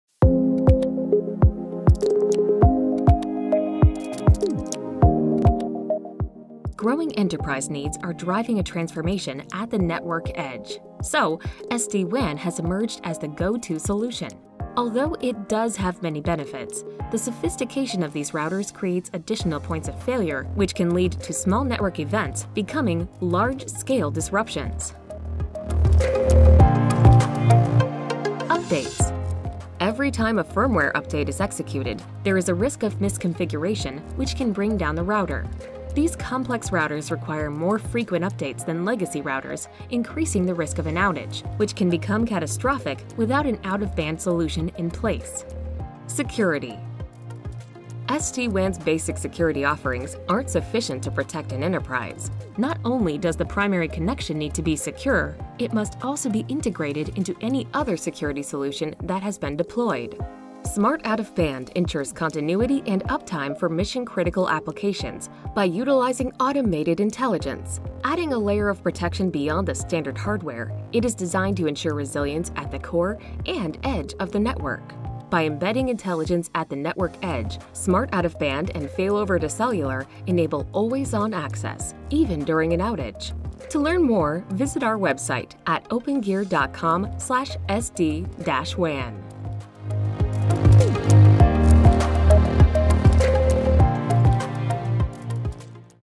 Smart Support For SD-WAN Deployments
SD-WAN is quickly becoming the standard in enterprise deployments because of its many benefits. However, before deploying, enterprises must understand points of failure that can occur with SD-WAN. Watch this video to learn how Smart Out-of-Band by Opengear ensures network resilience in the event of a disruption. Smart deployments start with Opengear.